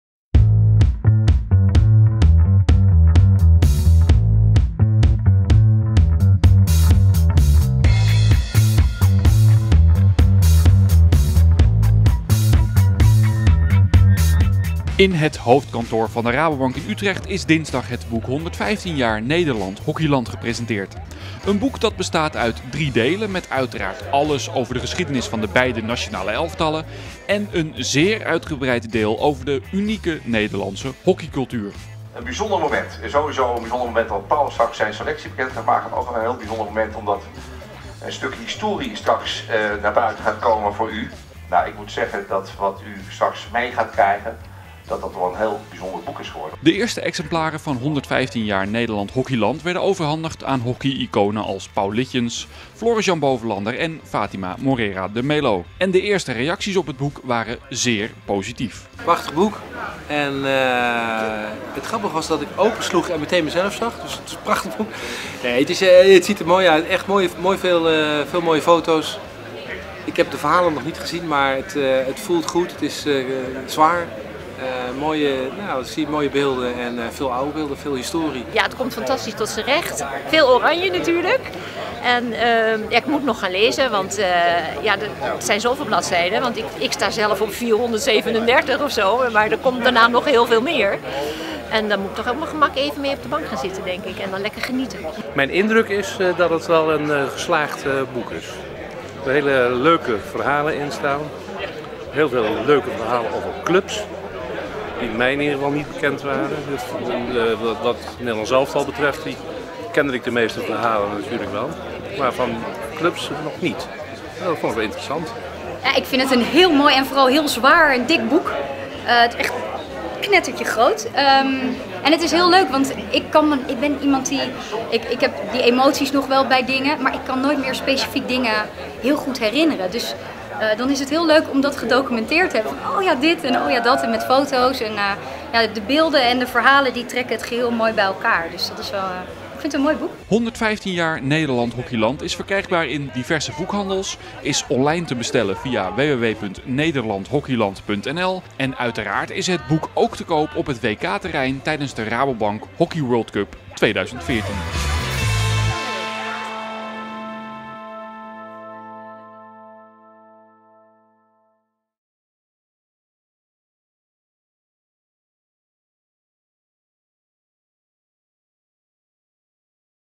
115 jaar Nederland Hockeyland
Met veel trots presenteren we u '115 jaar Nederland Hockeyland'. Een document waaraan ruim twee jaar is gewerkt door een reeks prominente hockeyjournalisten. Ter gelegenheid van het 115-jarig bestaan van de Koninklijke Nederlandse Hockey Bond (oktober 2013) en de Rabobank Hockey World Cup 2014 in Den Haag is, in opdracht van de KNHB, het boek geproduceerd door uitgeverij Arko Sports Media.

'115 jaar Nederland Hockeyland' bestaat uit drie delen. Twee delen gaan over de prestaties en belevenissen van de Oranje Dames en Heren. Met verrassende verhalen, die veelal nog nooit of althans niet zo uitgebreid aan het papier waren toevertrouwd. In het derde deel van het boek staat onze unieke hockeycultuur centraal, waarin alle 320 Nederlandse verenigingen staan vermeld.